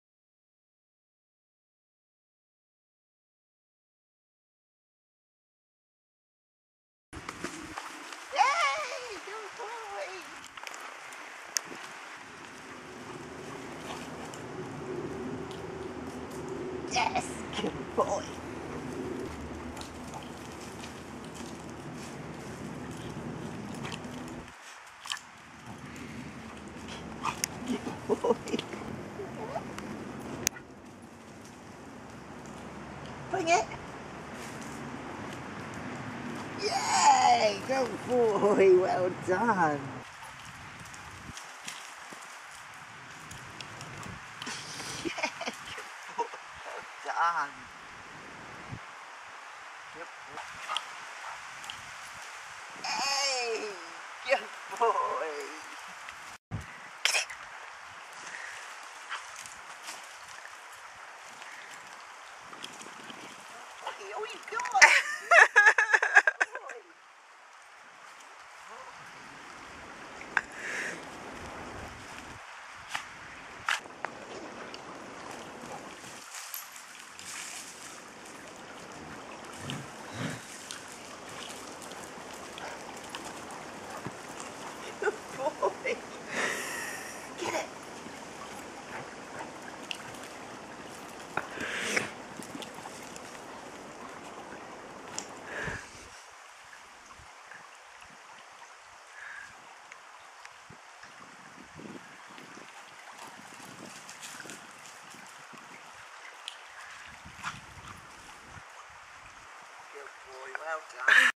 Fluke playing at woodlands in our clicker training place for hot days
We use this place to do clicker training on hot days because of the shade and access to running water.         Here is the latest game we have been teaching the dogs to play here.       Shot with a CANON POWERSHOT SX210 IS camera in HD mode.  Edited with a Dell Core I5 running WINDOWS 7